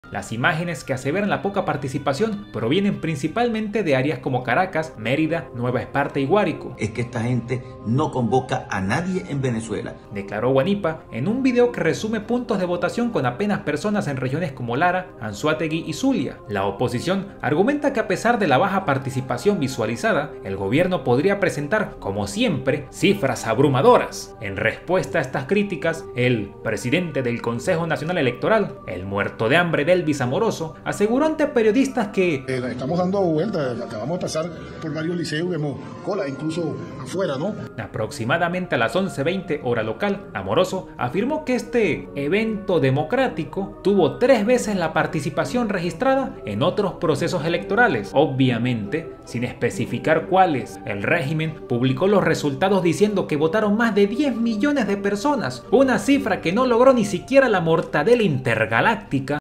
CNE anuncia más de 10 millones de “votos” en referéndum por el Esequibo
Varios partidos y políticos opositores, incluyendo Voluntad Popular (VP) y el exdiputado Juan Pablo Guanipa de Primero Justicia, han expresado su preocupación a través de las redes sociales, señalando imágenes de centros electorales vacíos o con poca afluencia. Las imágenes que aseveran la poca participación, provienen principalmente de áreas como Caracas, Mérida, Nueva Esparta y Guárico.
"Esta gente no convoca a nadie en Venezuela", declaró Guanipa en un video que resume puntos de votación con apenas personas en regiones como Lara, Aragua, Falcón, Anzoátegui y Zulia. La oposición argumenta que, a pesar de la baja participación visualizada, el Gobierno podría presentar cifras de participación más optimistas.
En respuesta a estas críticas, el presidente del Consejo Nacional Electoral (CNE), Elvis Amoroso, aseguró ante periodistas que ha observado "colas" en los centros de votación, incluso con gente esperando fuera. Aproximadamente a las 11:20 hora local, Amoroso afirmó tener 3 veces la participación registrada en otros procesos electorales, sin especificar cuáles.
A pesar de la discrepancia en las observaciones sobre la participación, la jornada electoral sigue su curso en los 15,857 centros habilitados. La consulta, aunque no implica un cambio inmediato sobre el área en disputa con Guyana, plantea la pregunta fundamental a los ciudadanos sobre la anexión del territorio del Esequibo al mapa nacional, un territorio que Venezuela no ha controlado desde 1899.